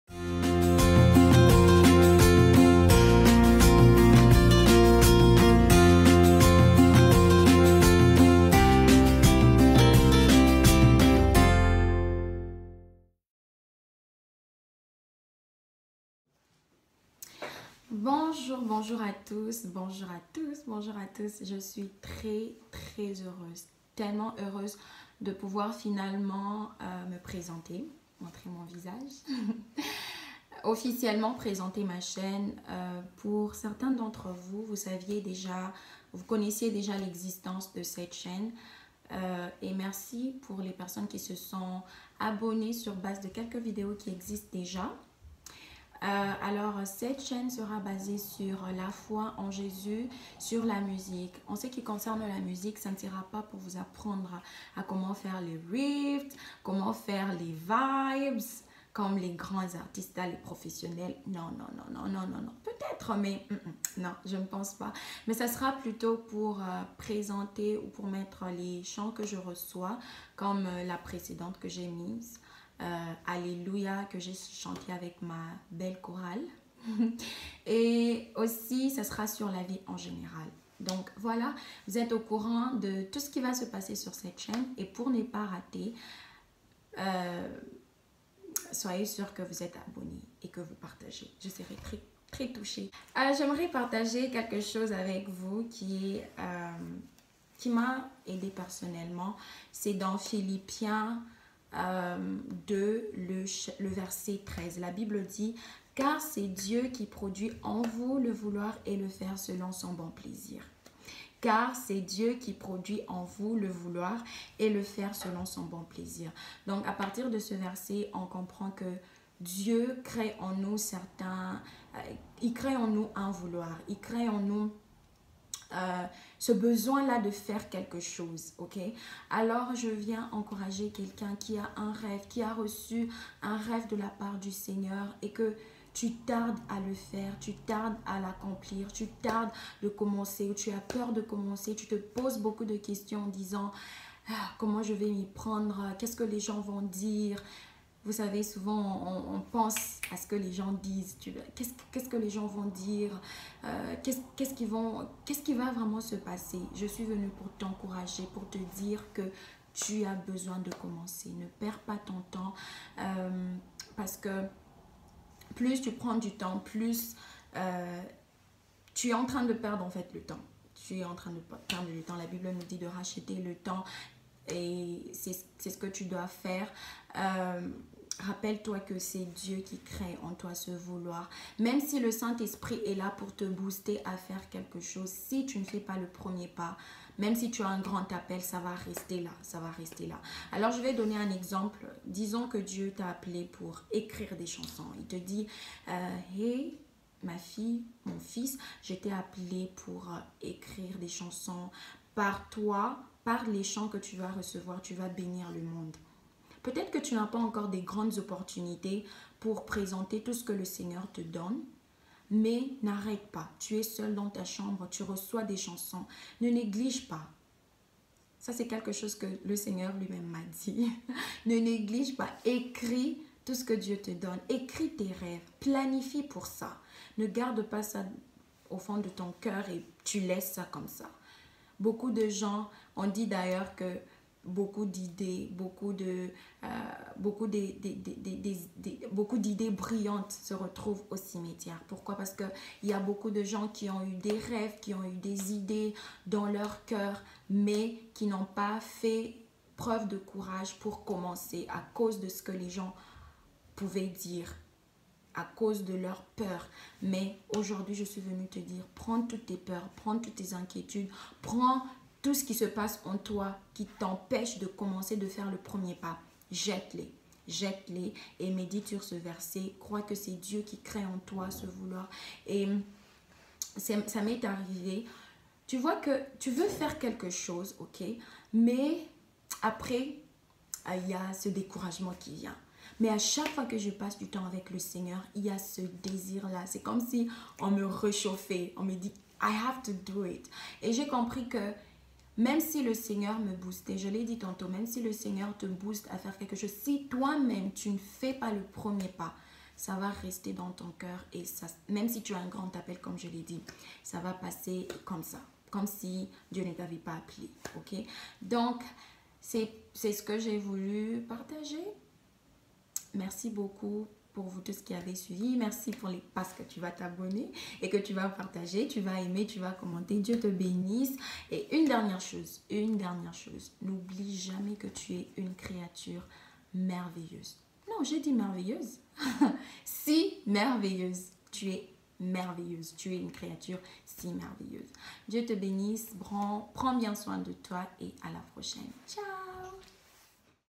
Sois Courageux (se)pour le rêve que tu as
Beaucoup de gens ont des grands rêves qui malheureusement restent rêves😔parceque on a  peur de commencer:Alors dans video d’introduction je vous invite à commencer ce rêve là sans perdre du temps